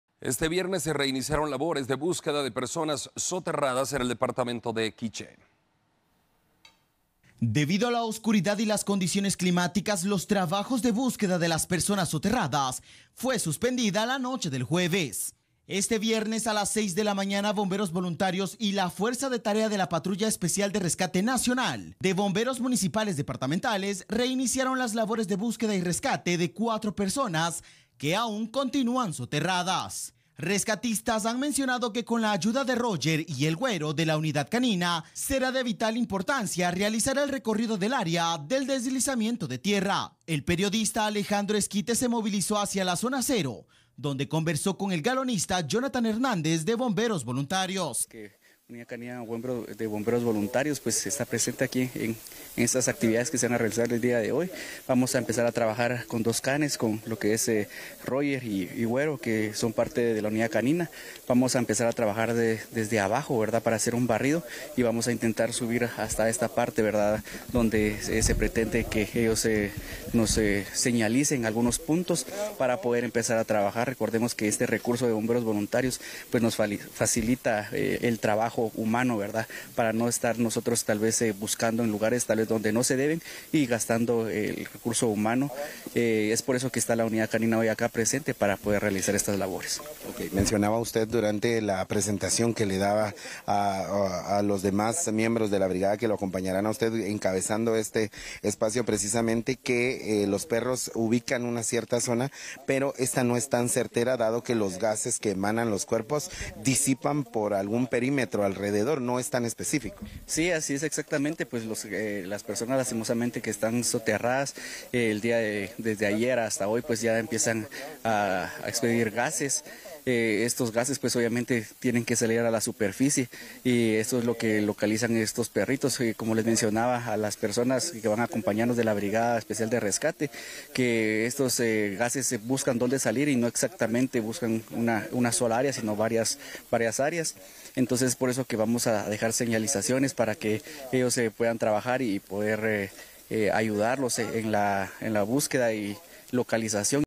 Labores de búsqueda se reanudaron en Quiche
Socorristas volvieron a los trabajos de búsqueda este viernes para poder localizar a las cuatro personas que quedaron soterradas en San Bartolomé Jocotenango, Quiché. Las labores se hicieron de forma manual. Además, la ayuda de Roger y El Güero, de la unidad canina, será de vital importancia.